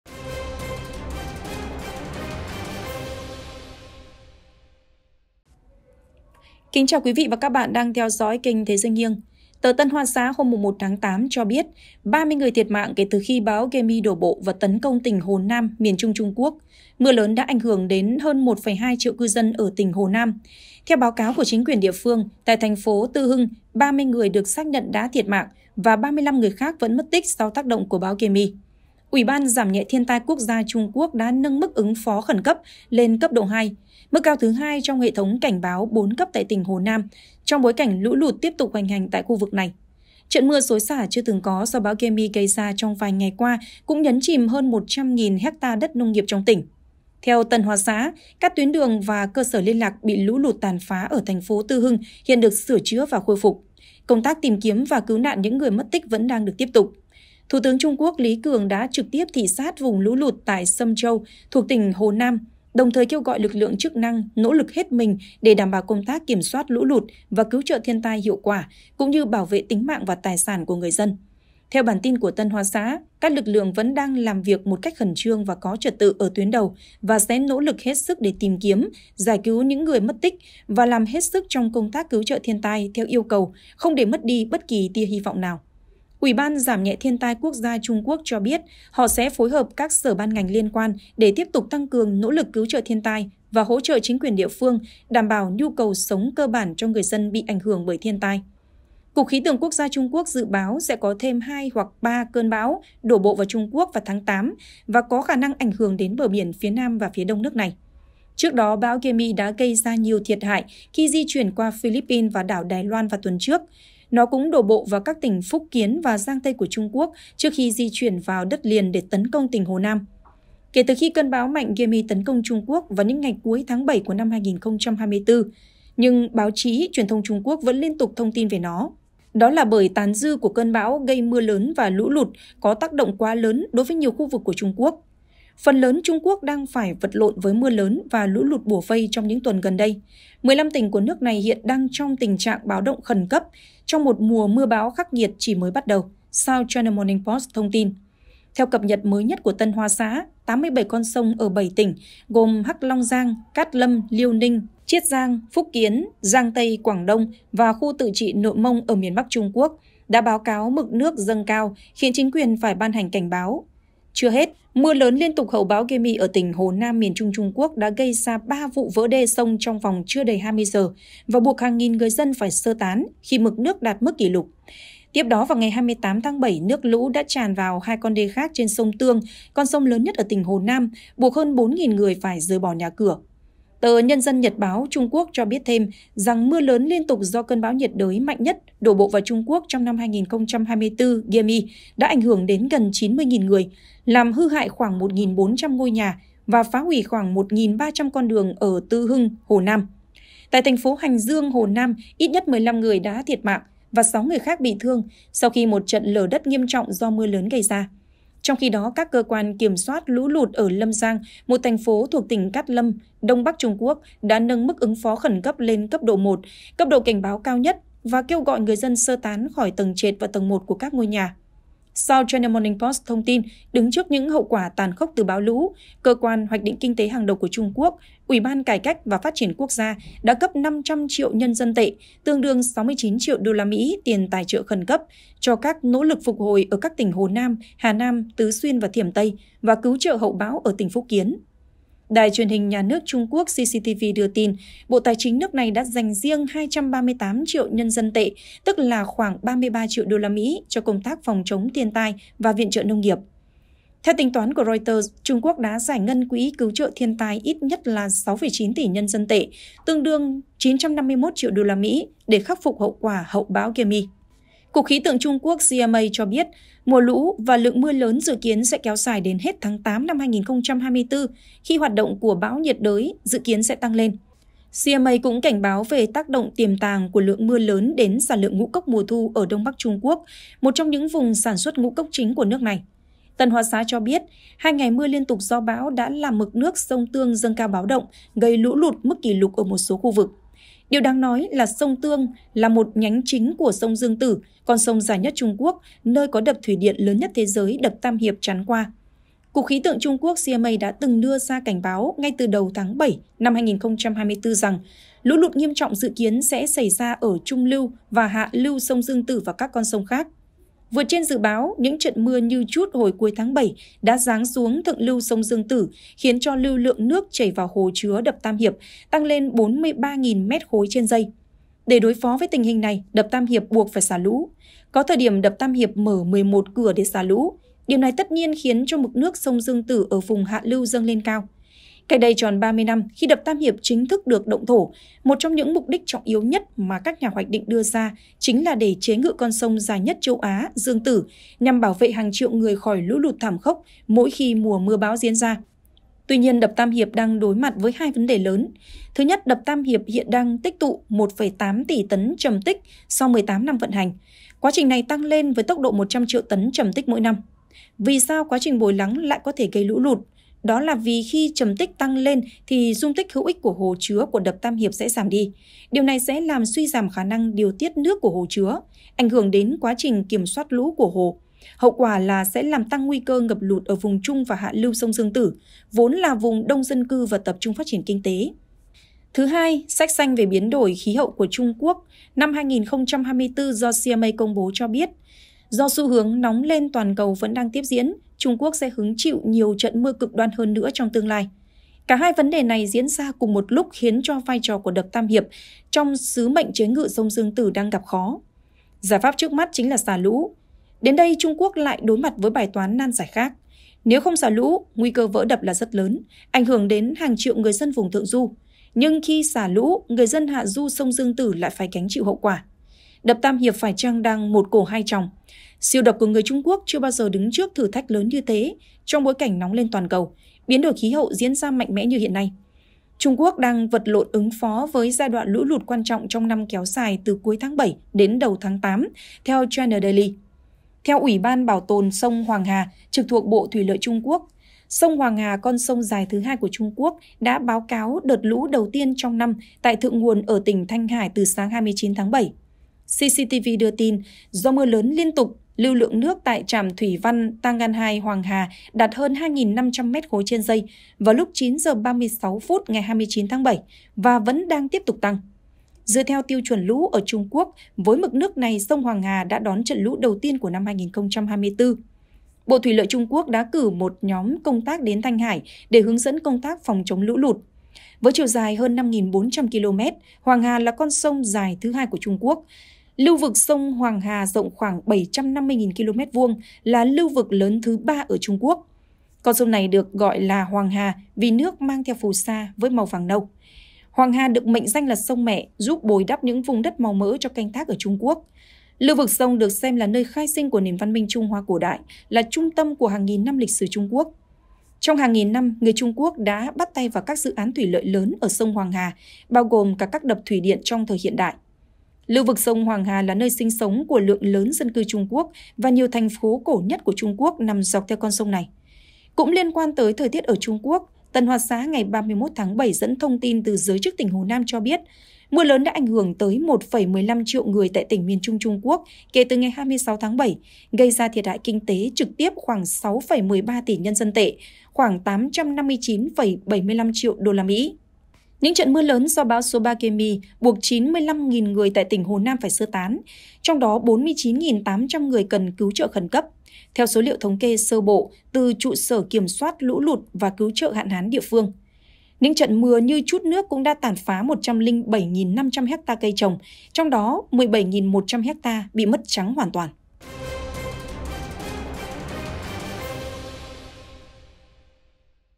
Trung Quốc lũ lụt kinh hoàng cuốn trôi hàng chục mạng người, hơn 1 triệu người ‘chìm’ giữa biển nước
THẾ GIỚI NGHIÊNG | Tờ Tân Hoa xã hôm 1/8 cho biết, 30 người thiệt mạng kể từ khi bão Gaemi đổ bộ và tấn công tỉnh Hồ Nam (miền trung Trung Quốc). Mưa lớn đã ảnh hưởng đến hơn 1,2 triệu cư dân ở tỉnh Hồ Nam, theo báo cáo của chính quyền địa phương. Tại thành phố Tư Hưng, 30 người được xác nhận đã thiệt mạng và 35 người khác vẫn mất tích sau tác động của bão Gaemi.

▶ THẾ GIỚI NGHIÊNG - Thông tin nhanh nhất, chính xác nhất